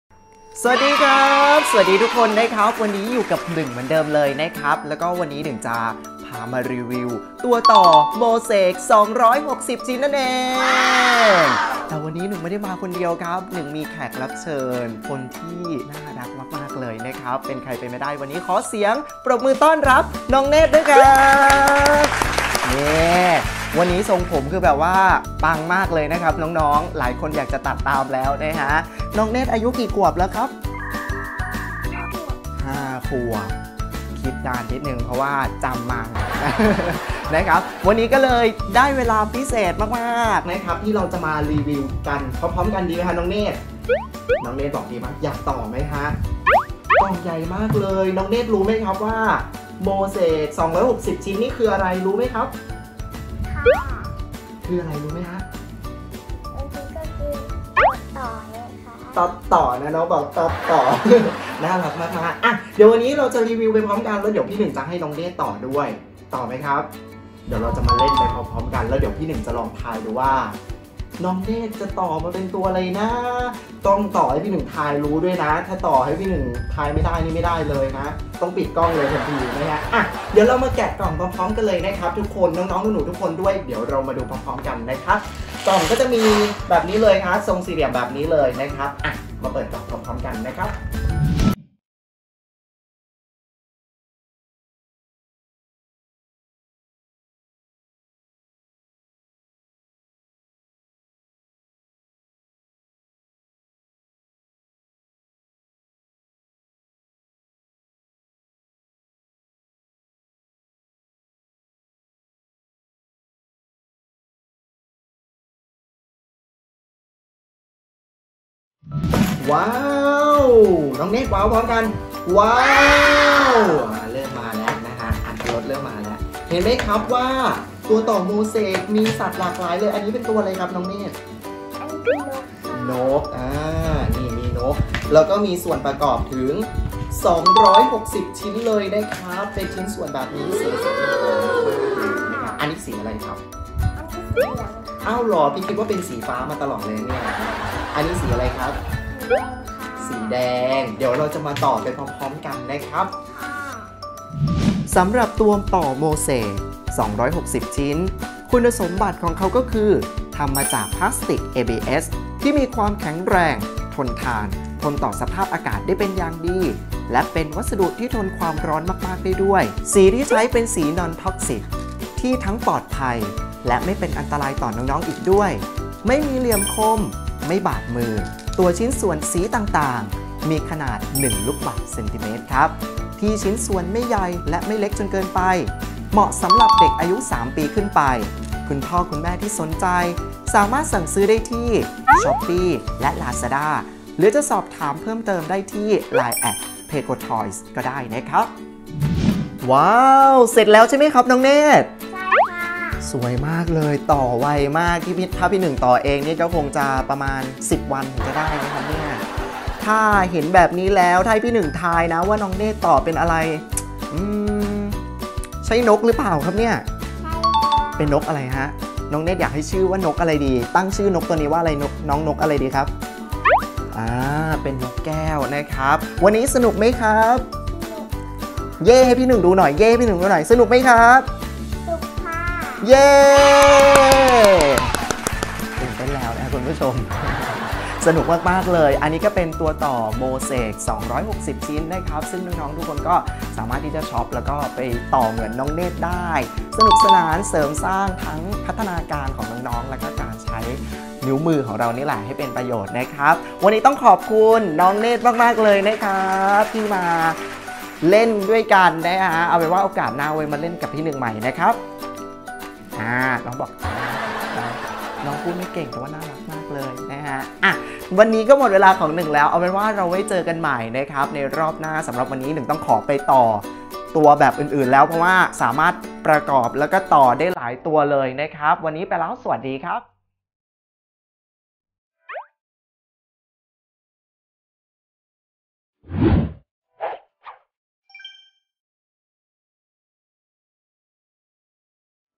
Playgotoys 2097 MOSAIC PUZZLE ART-OVER ของเล่นเสริมพัฒนาการเด็กจากฮ่องกง มาตรฐานยุโรป
ตัวต่อโมเสค260ชิ้น ของเล่นเสริมพัฒนาการ Playgotoys 2097 MOSAIC PUZZLE ART-OVER 260 PCS

เป็นของเล่นสำหรับเด็กเล็ก อายุตั้งแต่ 3 ปีขึ้นไป ทำจากพลาสติกที่แข็งแรงทนทาน ไม่แตกหักง่าย สามารถช่วยให้น้อง ๆ สนุกกับการจินตนาการ เพิ่มความคิดสร้างสรรค์ ซึ่งจะนำไปสู่ประโยชน์มากมาย ในการกระตุ้นและเสริมศักยภาพ ด้านสมองได้เป็นอย่างดี

สินค้า Playgotoys เป็นของเล่นที่ทำจากพลาสติกเกรด A ใช้สี Non-Toxic ซึ่งไม่มีอันตรายต่อเด็ก สีสันสดใสดูแล้วสบายตา ไม่มีเหลี่ยมคม เหมาะสำหรับเด็กอายุ 3 ปีขึ้นไป

★ ตัวต่อโมเสค260ชิ้น
1. สนุกสนานไปกับการประกอบเป็นรูปต่าง ๆ
2. เสริมพัฒนาการทางจินตนาการ
4. ไร้เหลี่ยมคม สีสันสดใส

★ ประโยชน์ที่จะได้รับ
✔️ ทำให้น้องสนุกสนานกับการทำกิจกรรมร่วมกับคุณพ่อคุณแม่
✔️ เสริมสร้างพัฒนาการในน้อง ๆ มีจินนตาการที่กว้างไกล

Good Trip : เหมาะสำหรับคุณแม่ที่ต้องการเสริมสร้างพัฒนาการลูกน้อยในช่วงวัย 3 ปีขึ้นไป สามารถสอนน้อง ๆ ในเรื่องของการฝึกกล้ามเนื้อมือ และ ความคิดสร้างสรรค์

มีหน้าร้านอยู่ที่
63/401 ซ.บรมราชชนนี 64
หมู่บ้านทิพย์มณฑล แขวงศาลาธรรมสพน์
เขตทวีวัฒนา กรุงเทพฯ. 10170